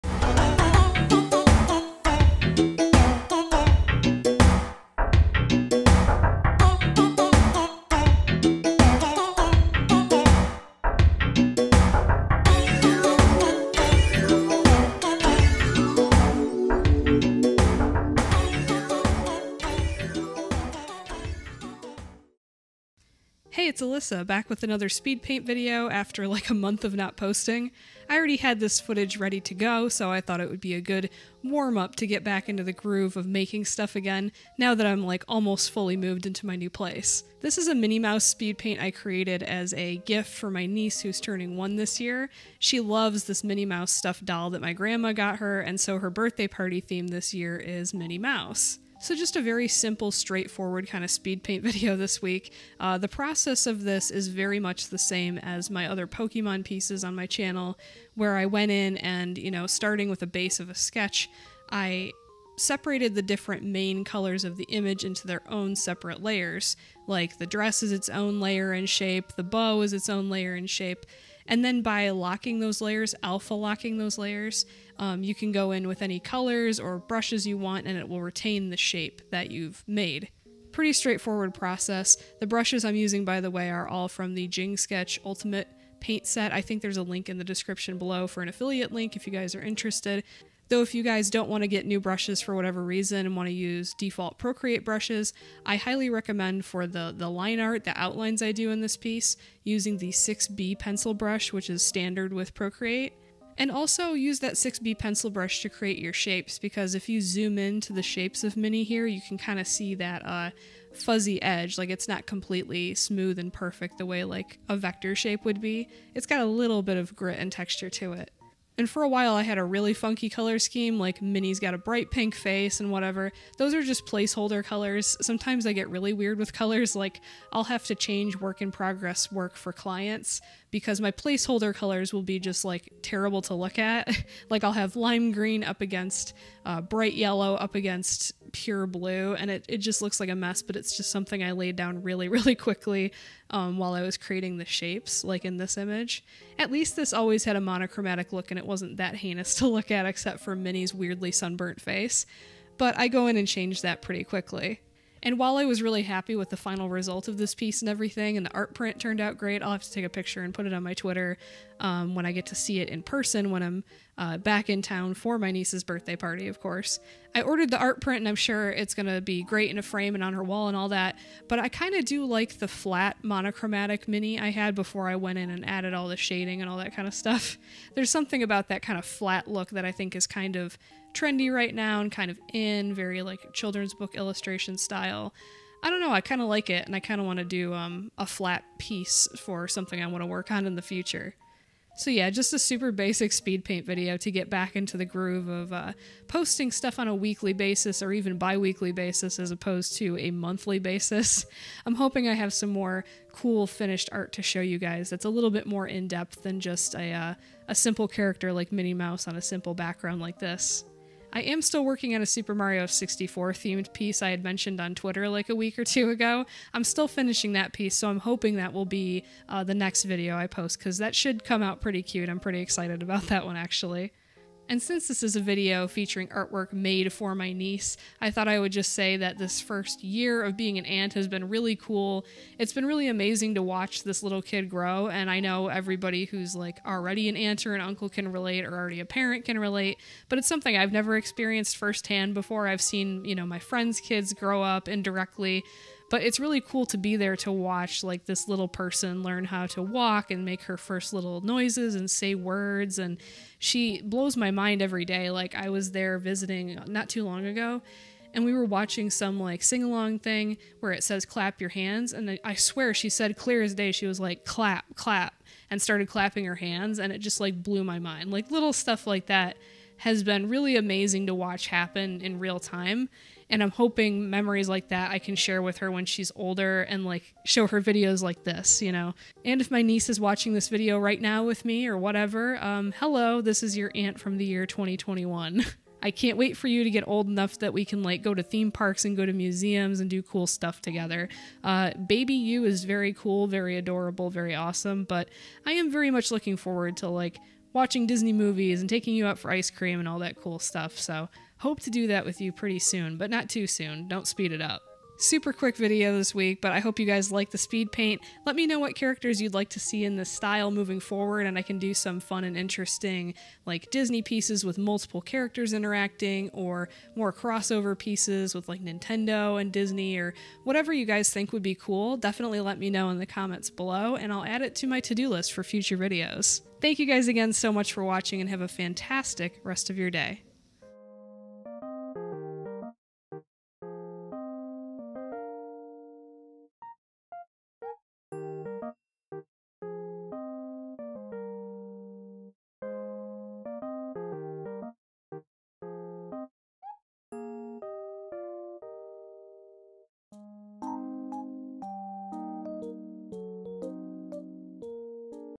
Minnie Mouse speedpaint | iPad Drawing Process Video
A quick Minnie Mouse speedpaint is a good warmup video for me to try and get back into a routine again, haha.

support me for free by signing up for my once a month (or less, lol) newsletter

intro/outro music kindly provided by

brushes most commonly used: Procreate 6b pencil, bonobo chalk, dry ink

software/hardware used:
iPad pro 12.9" with Apple Pencil
Astropad Pro
Procreate